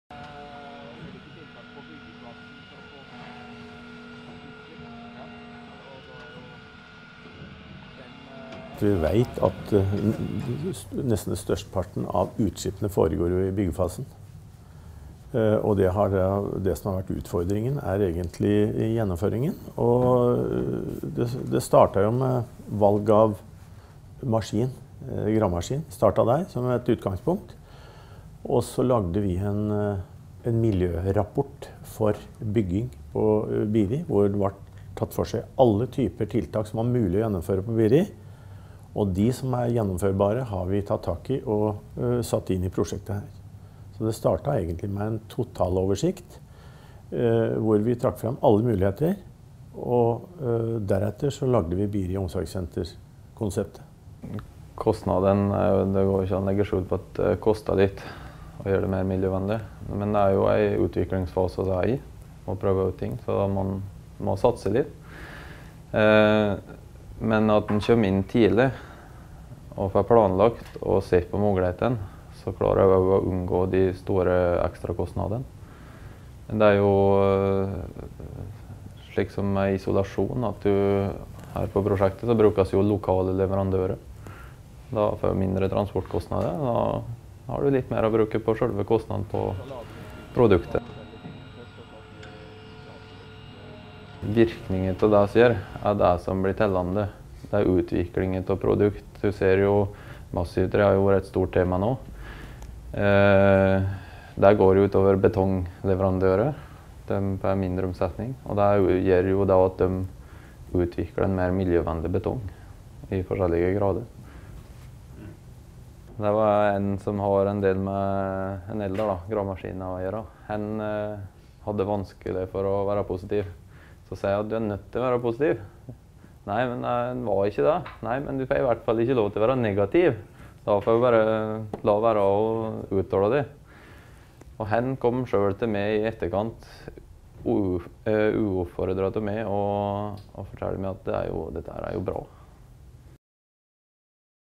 Elektrisk gravemaskin på Gjøvik
Ved hjelp av både elektrisk gravemaskin og tårnkran har Gjøvik kommune gjort store innhogg i klimagassutslippene under byggingen av nye omsorgsboliger på Biri. Kommunen hadde høye klimaambisjoner for det nye bygget nord for Gjøvik. Les mer på